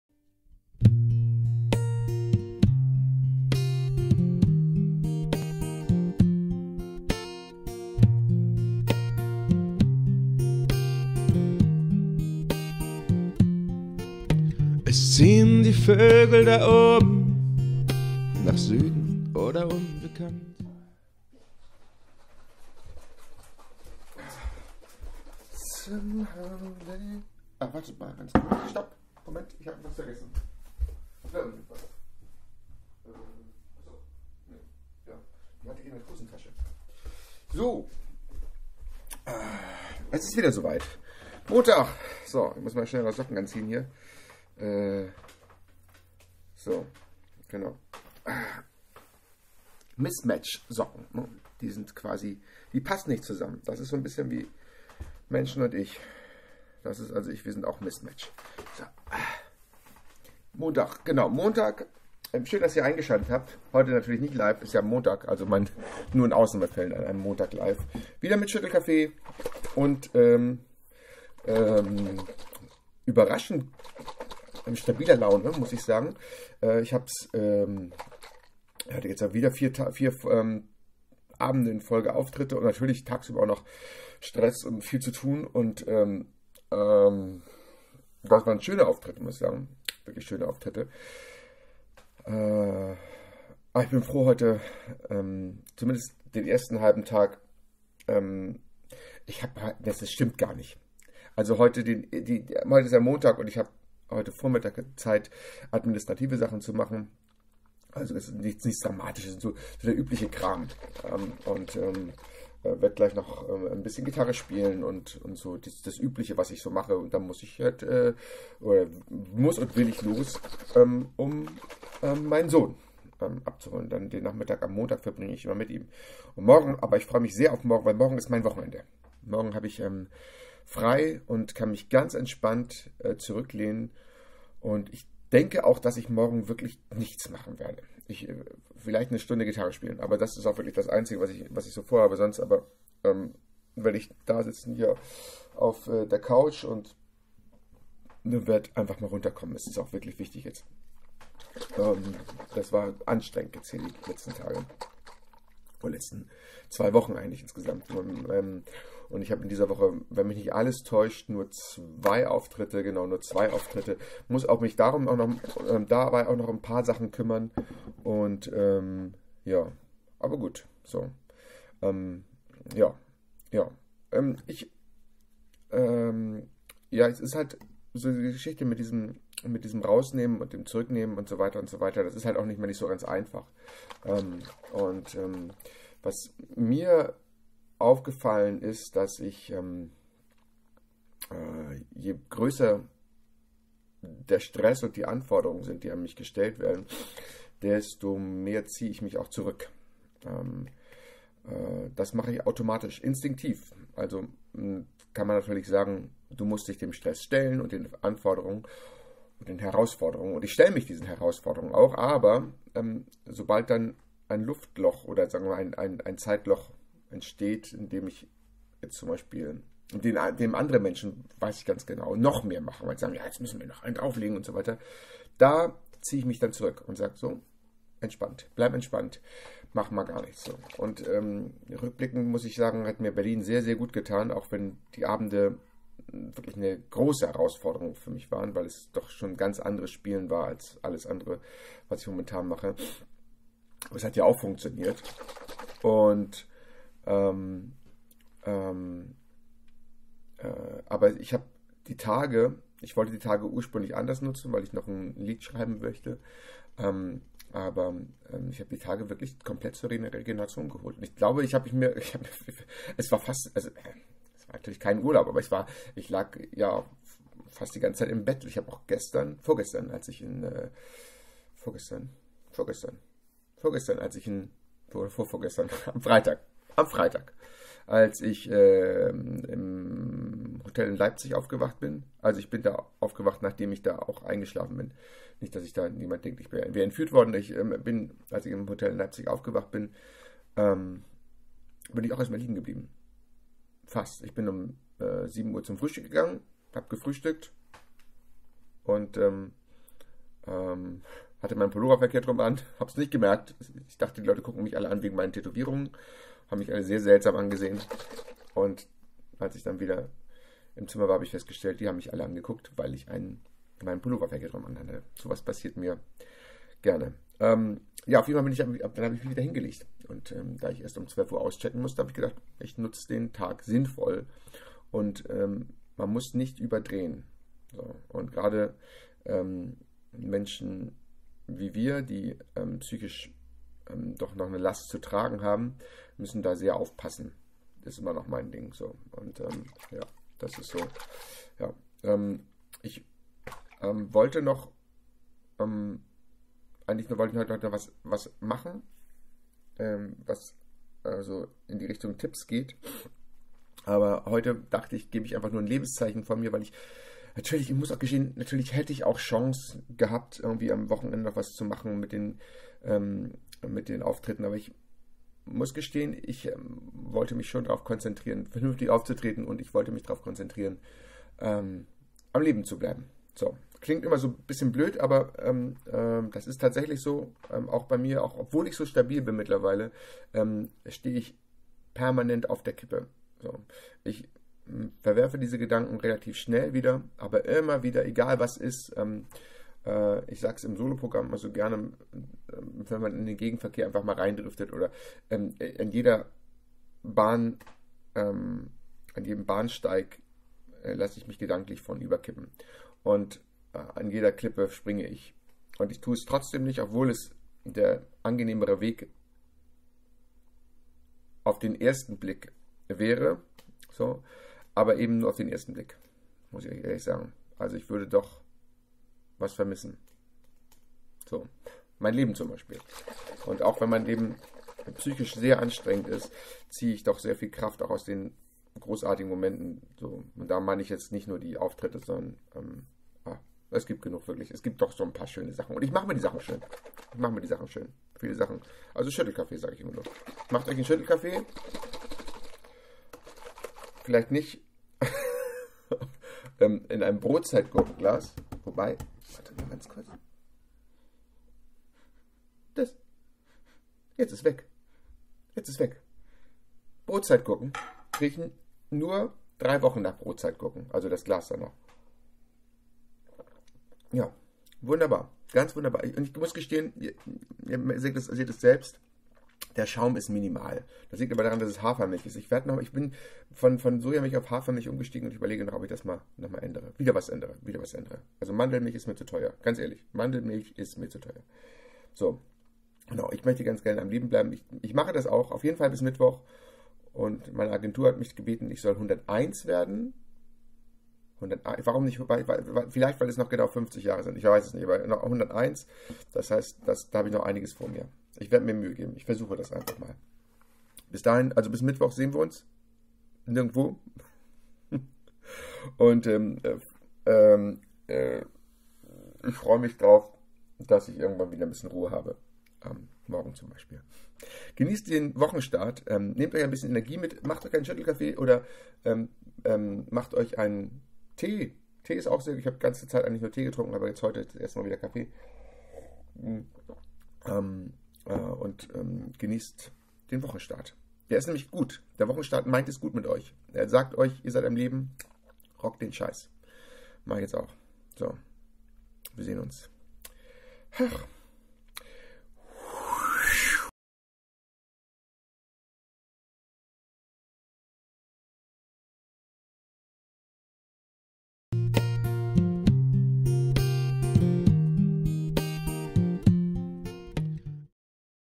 Depressionen und: Immer noch stabil
Montag. Es geht mir immer noch nicht schlecht, was ja auch ganz gut ist.

Es ist ja nicht so einfach, sich selbst zu helfen. Und wenn mir das gelingt, muss das nicht auf andere zutreffen.